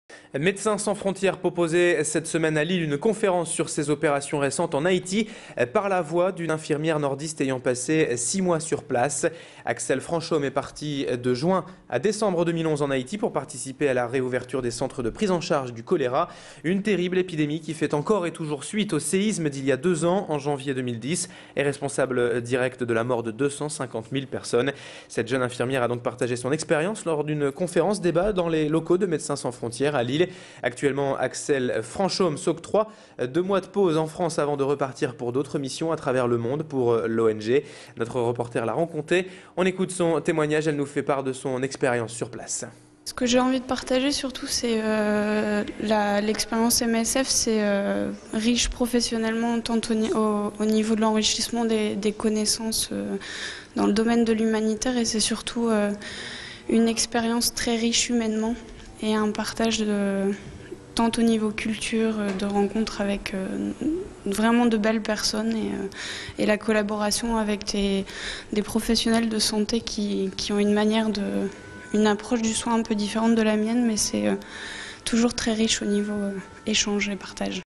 Medecins sans Frontières : L'action en Haïti
A Lille, Axelle Franchomme, une jeune infirmière de Médecins sans Frontières, tenait une conférence pour témoigner des actions de l'ONG en Haïti

Sites webs/blogs, copiez cette vidéo avec le bouton "Intégrer" !